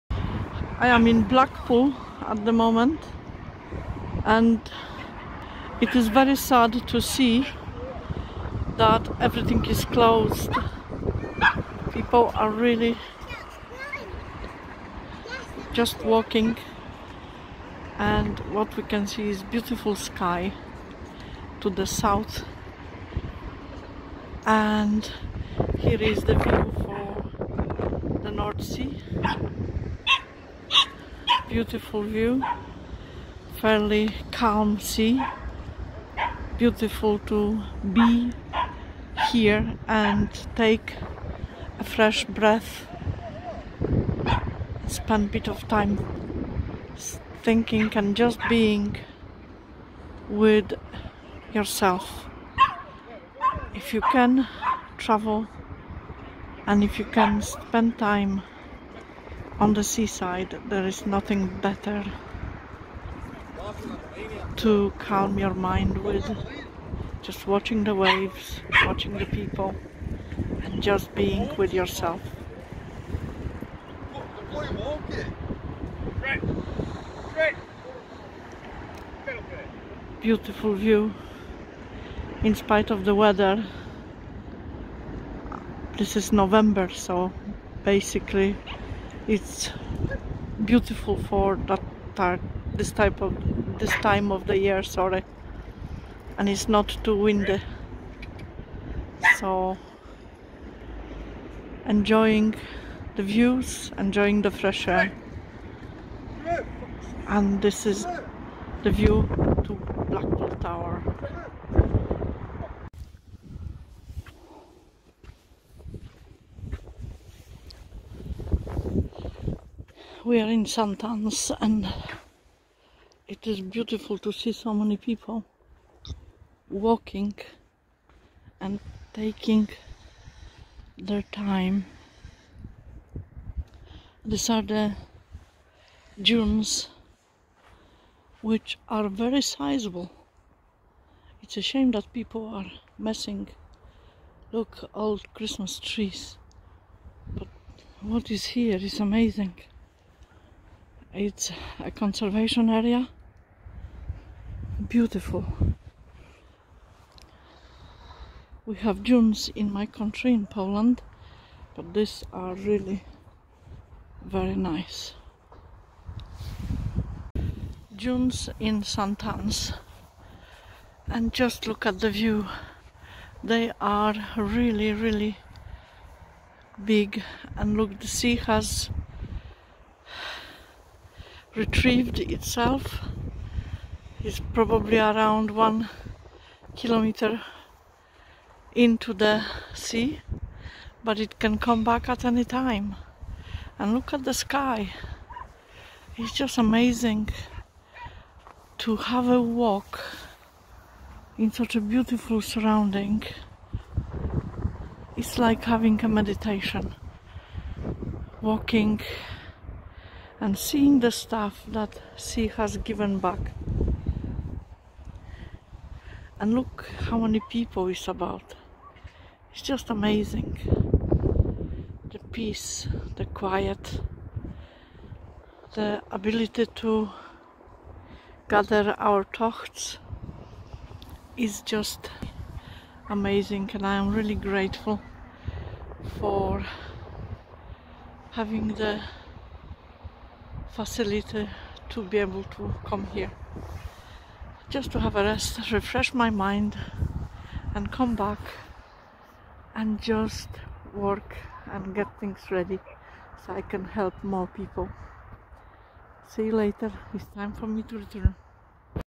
The Importance Of Being Whole...
I don`t do this too often, but I have decided to it more often. Denying a time for myself was a mistake. Having the ability to take time off and spend it at the seaside or in a forest is a must.
There is a time, when one must consider own health and take time to travel to quiet place and recover thoughts, clear mind and breath in the most amazing fresh air.
Where better to go than to St Anns , to the Dunes, have a walk on the beach, or simply walk the trails on the sand and simply be.